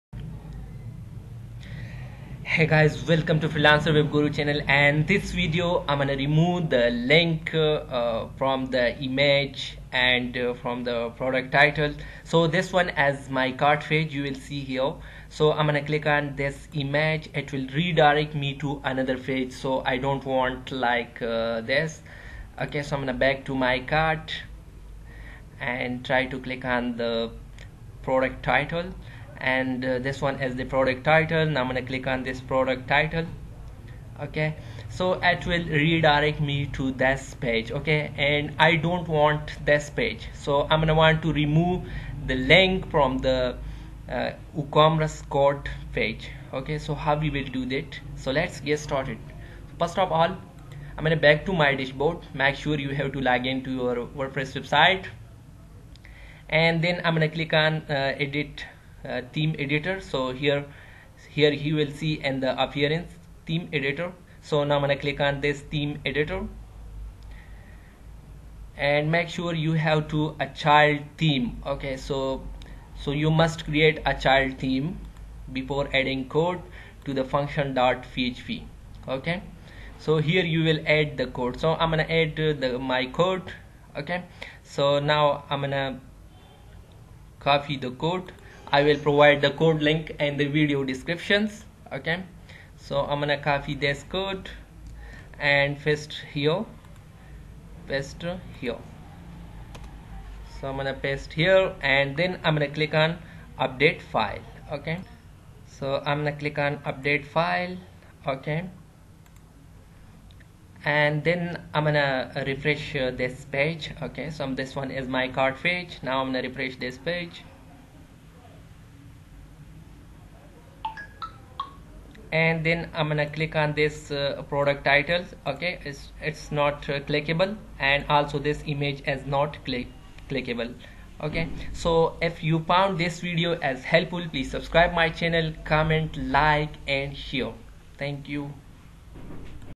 How to Remove Cart Page Link From Woocommerce Wordpress
/*hide cart page links image title*/
add_filter( 'woocommerce_cart_item_permalink', '__return_null' );